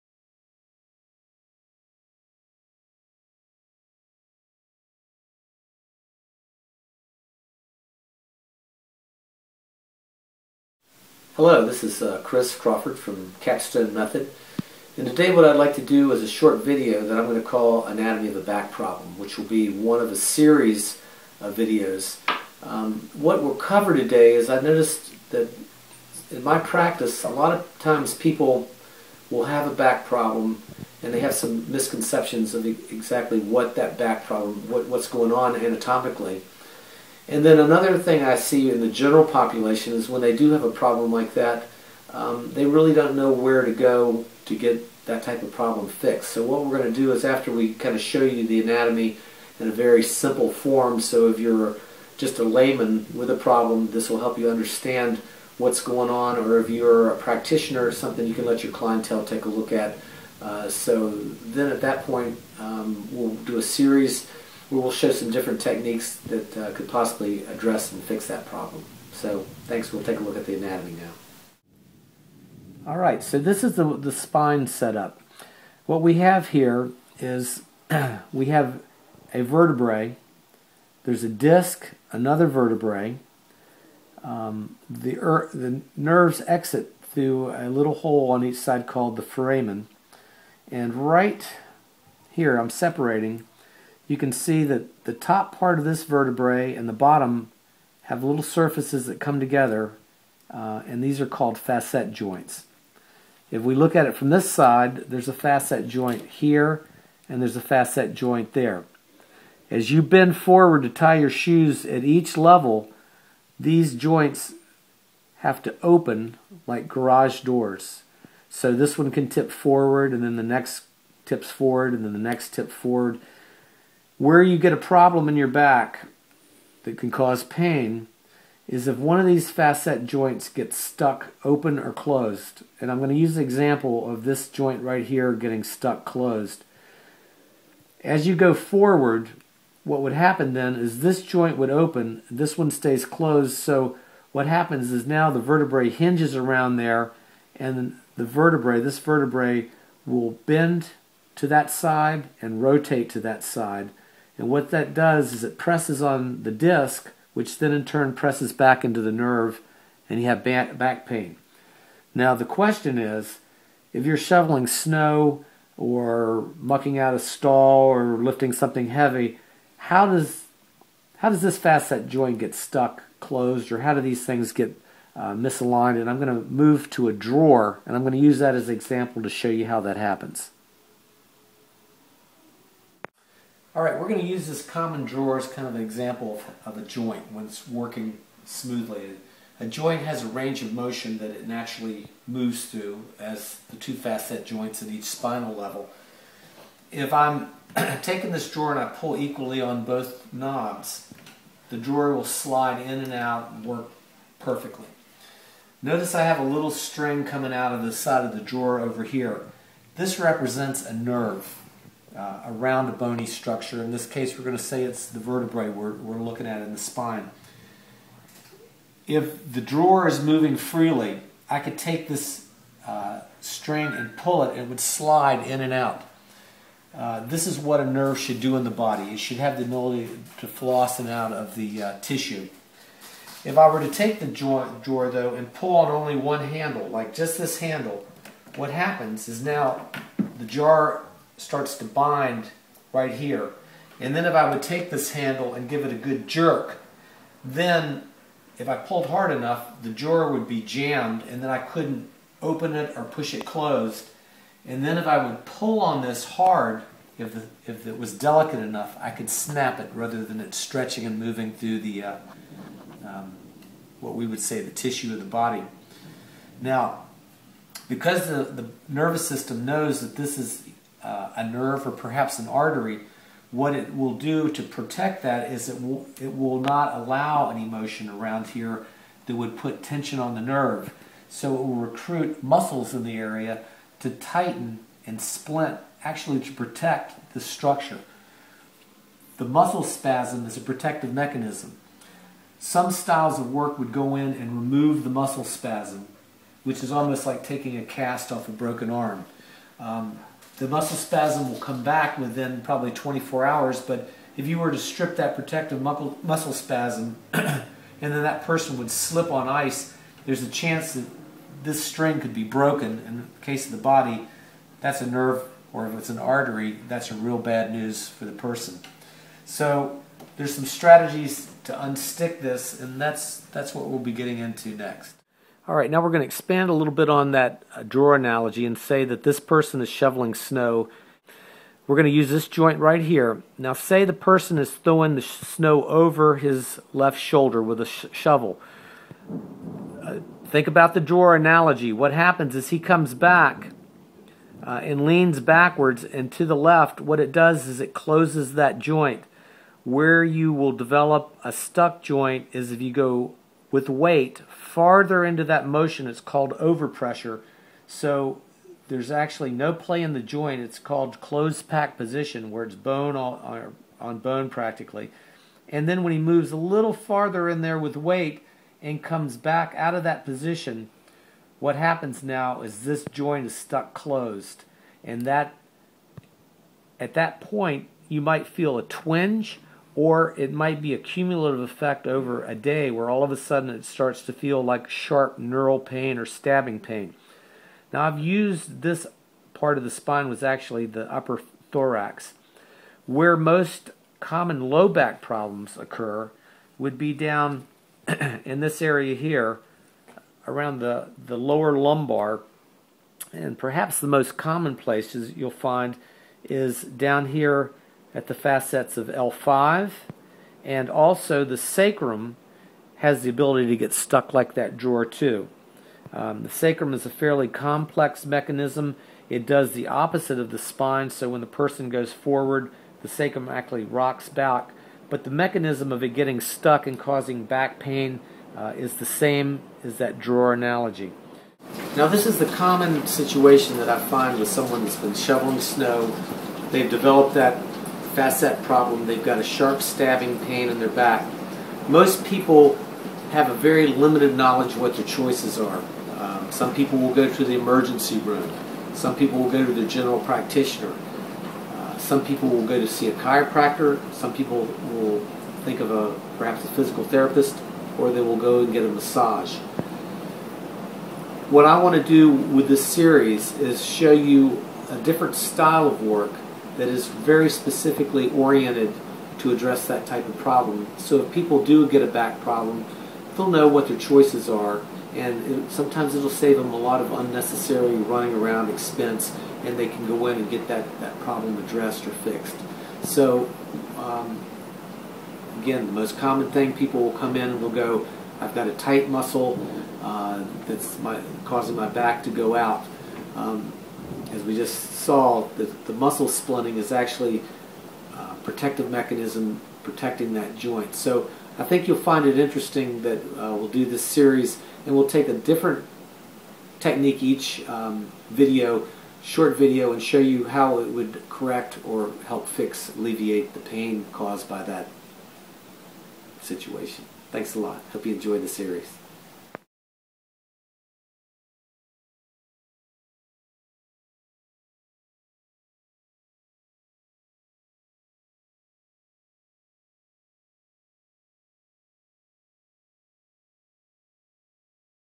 Back Pain? Anatomy of Back Pain Explained
Back pain? If you've ever wondered about what causes back stay tuned as Chris Crawford shares his perspective on the mechanics within the human body that leads to back pain and other structural problems.

*A note from us on the video quality: We know you're used to our higher quality videos but we thought you'd appreciate the content of this video anyway!